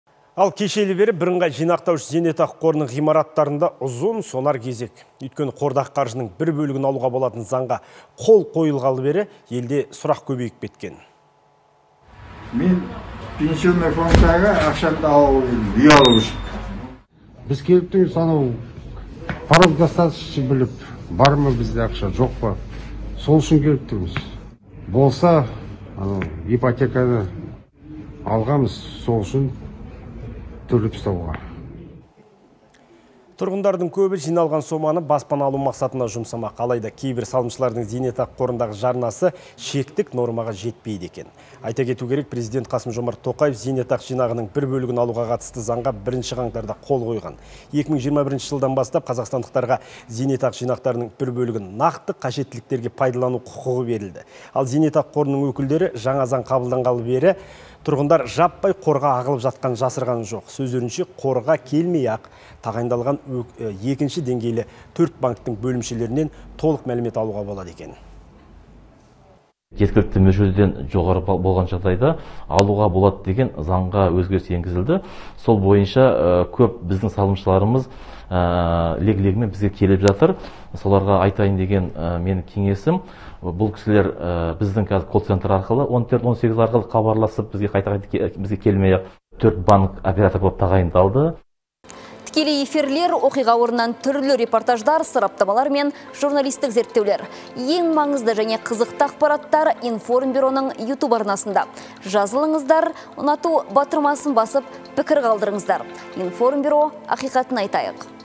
Заң қабылданғалы зейнетақы қорынан ақша шешуге талпынған елдің сұрағы көп
Кешелі бері Бірыңғай Жинақтаушы зейнетақы қорының ғимараттарында ұзын-сонар кезек. Өйткені қордағы қаржының бір бөлігін алуға болатын заңға қол қойылғалы елде сұрақ көбейген.
Тұрғындардың көбі жиналған соманы баспана алу мақсатына жұмсамақ. Алайда, кейбір салымшылардың зейнетақы қорындағы жарнасы шектік нормаға жетпейді. Айта кету керек, Президент Қасым-Жомарт Тоқаев зейнетақы жинағының бір бөлігін алуға қатысты заңға 1 қаңтарда қол қойған. 2021 жылдан бастап қазақстандықтарға зейнетақы жинақтарының бір бөлігін нақты қажеттіліктерге пайдалану құқығы берілді. Ал зейнетақы қорының өкілдері жаңа заң қабылдалғалы бері тұрғындар жаппай қорға ағылып жатқанын жасырмады. Сөздерінше, қорға келмей-ақ тағайындалған екінші деңгейлі төрт банктің бөлімшелерінен толық мәлімет алуға болады.
МАМЫРАЛЫ МАМБЕТОВ, «БЖЗҚ» АҚ Қарағанды  директоры:

– Жеткілікті мөлшерден жоғары болған жағдайда алуға болады деген заңға өзгеріс енгізілді. Сол бойынша, көп біздің салымшыларымыз лег-легімен бізге келіп жатыр. Айтайын деген кеңесім, бұл кісілер біздің ккол центр арқылы 1418 хабарласып, бізге қайта-қайта келмей-ақ, 4 банк оператор болып тағайындалды.